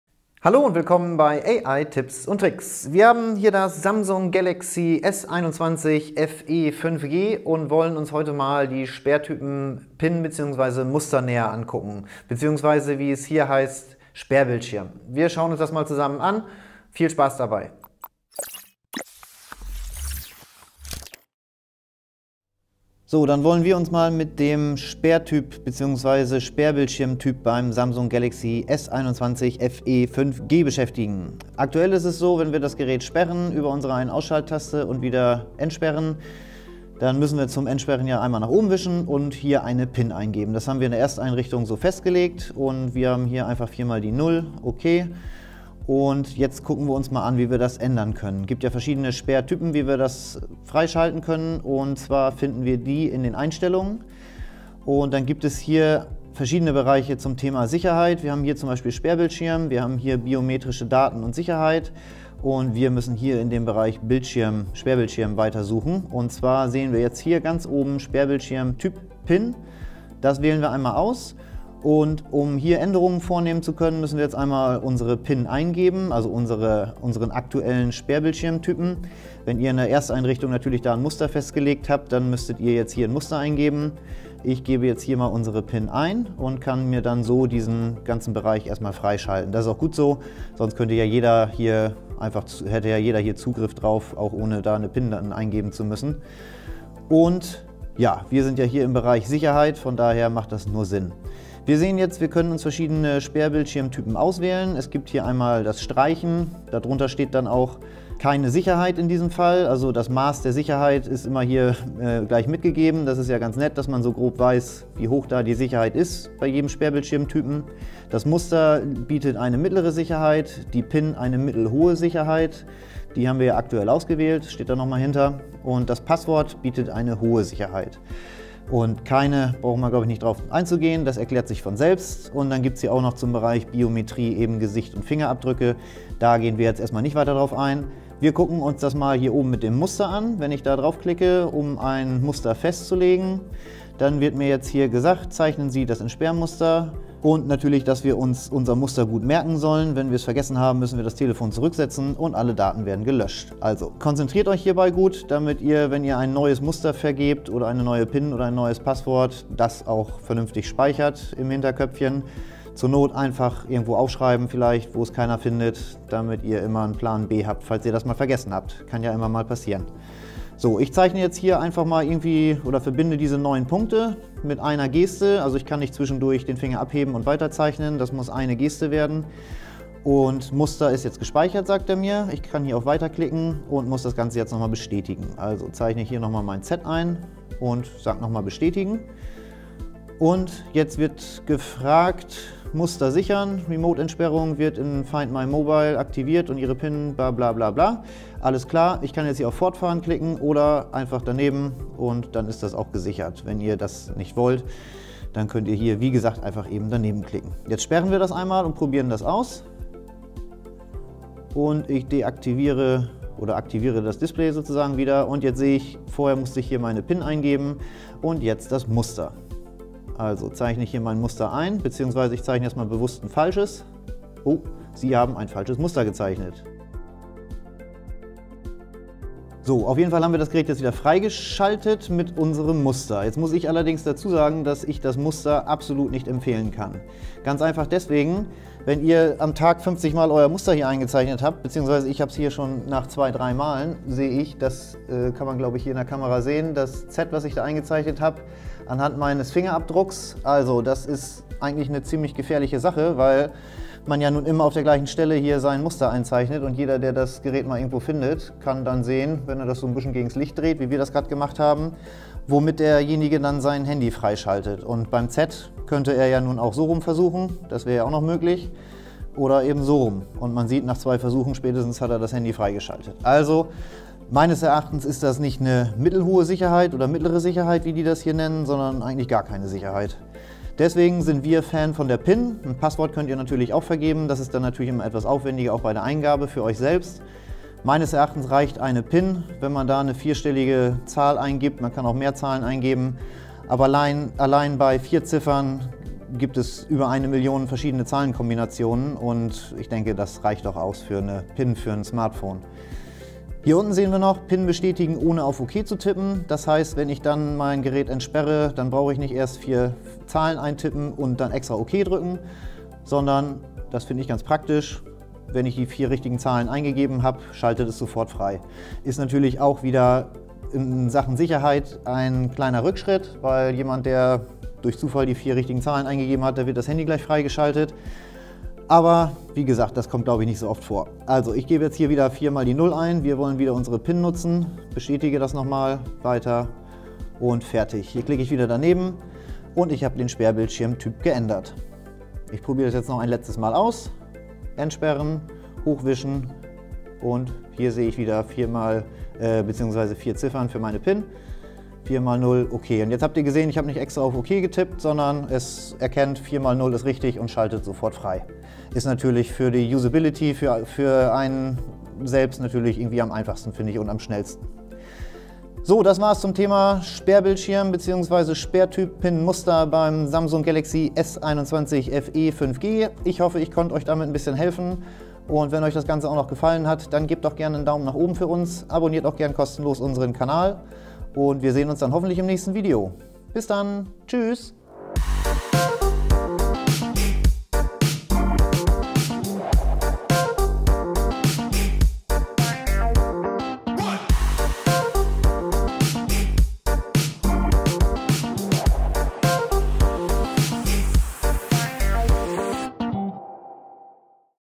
Samsung Galaxy S21 FE 5G - Sperrtyp PIN und Muster • 📱 • 🀡 • ☡ • Anleitung | Tutorial
Wir zeigen Euch, wie Ihr am Samsung Galaxy S21 FE 5G den Sperrtyp PIN und Muster anwenden könnt und damit Euer Display entsperrt. Sofern Ihr bei Eurer Ersteinrichtung Eures S21 FE bereits eine gute PIN vergeben habt, habt Ihr ja schon für den notwendigen Schutz Eures Smartphones gesorgt. Falls nicht, oder Ihr eine andere PIN eingeben wollt, oder das Muster nutzen wollt, dann ist unser Video hoffentlich genau das Richtige für Euch. Wir zeigen Euch hier, wie man eine Displaysperre einrichtet bzw. den Sperrtyp ändern kann.

Unser Tipp:
Mit einer vierstelligen PIN habt Ihr ausreichenden Schutz. Unsere beispielhaften Anwendungen von PINs wie "0000" oder "1234" sind nur zur Demonstration und sollten nicht wirklich verwendet werden, da sie kaum Schutz bieten (die werden oft als erstes probiert, um in ein fremdes Telefon zu gelangen). Schutzfunktionen wie Muster oder auch das Entsperren mit dem Gesicht bieten keinen ausreichenden Schutz. Bei einem Passwort, das aus Zahlen-Buchstaben-Kombinationen besteht und auch noch mehr als vier Zeichen beinhaltet wird man wohl irgendwann genug davon haben, das immer eingeben zu müssen, da es weit aufwendiger als eine gut überlegte (und gemerkte) PIN ist.

FunFact:
Bei einer vierstelligen PIN gibt es bei Verwendung der Zahlen 0 - 9 insgesamt 10000 verschiedene Möglichkeiten - und man hat ja immer nur drei Versuche am Stück 😉